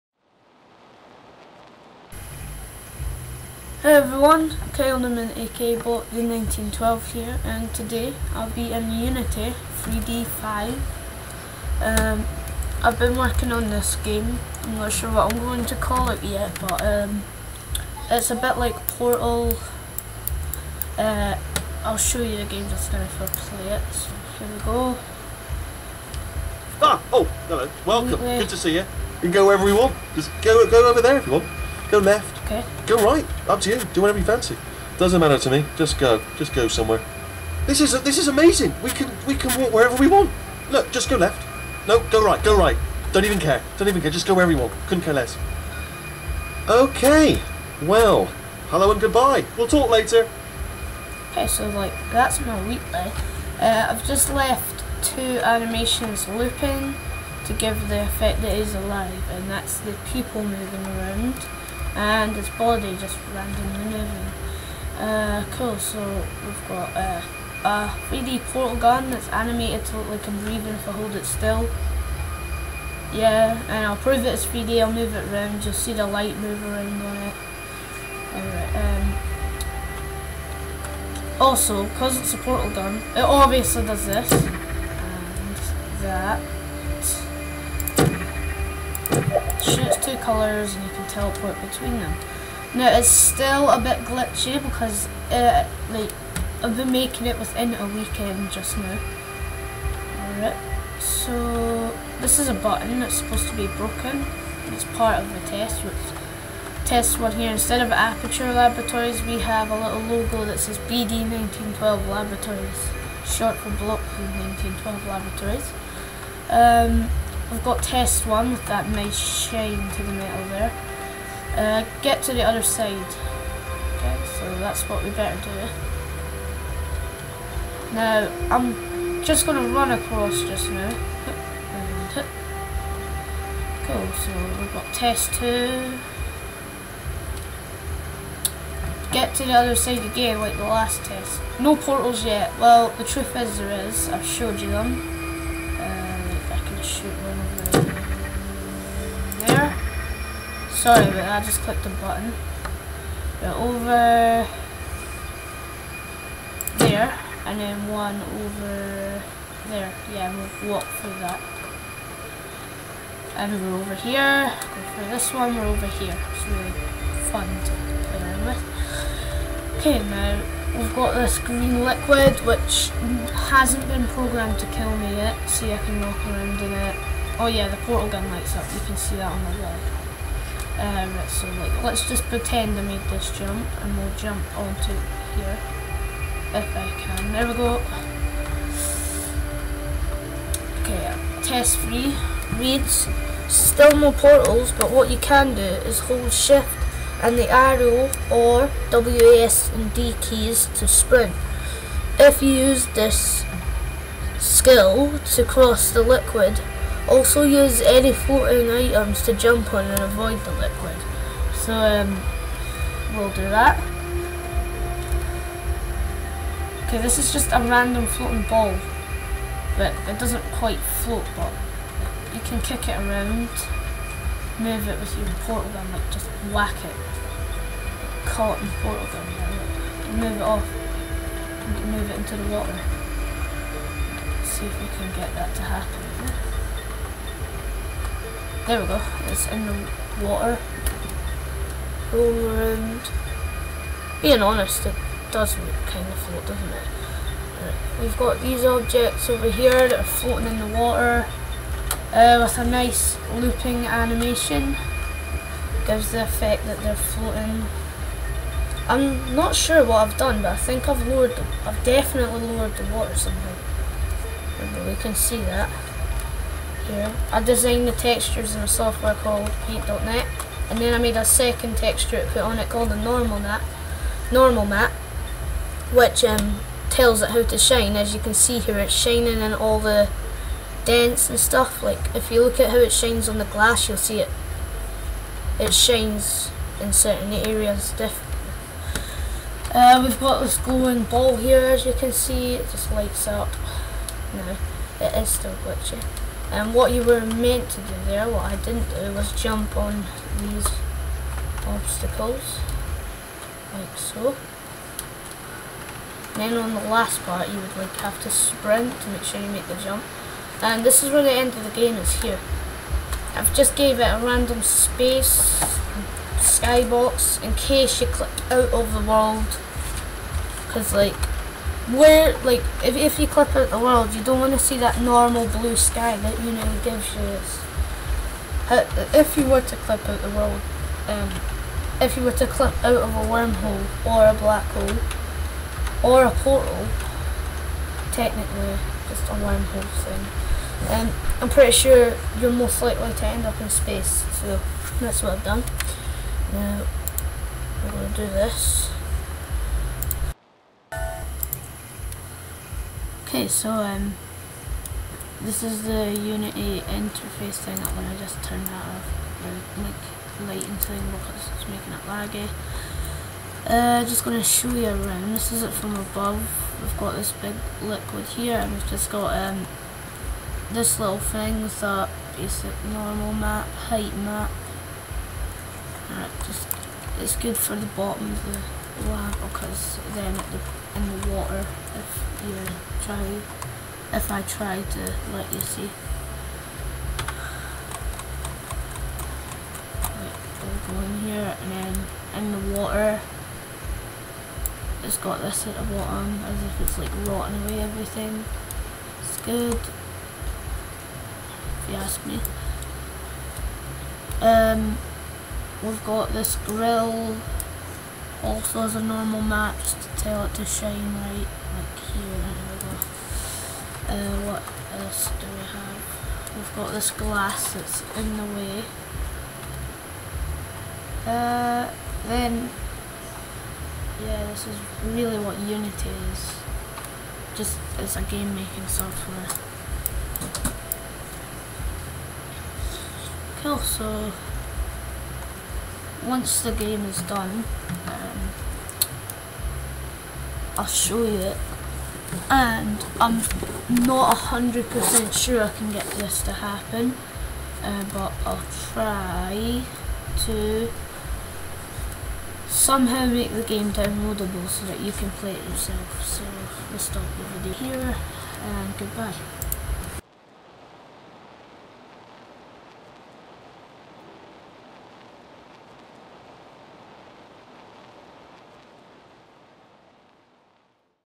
Portal like Game in Unity3D5 by Kyle Newman
Hello YOUTUBE ! Today I want to show you my Game I made in UNITY3D5 .As you can see it's based on the game (Portal 2) its not complete yet but when it is I will put another video up ,and I'll try to put my game up for download (((FREE))) but I can't promiss you this will happen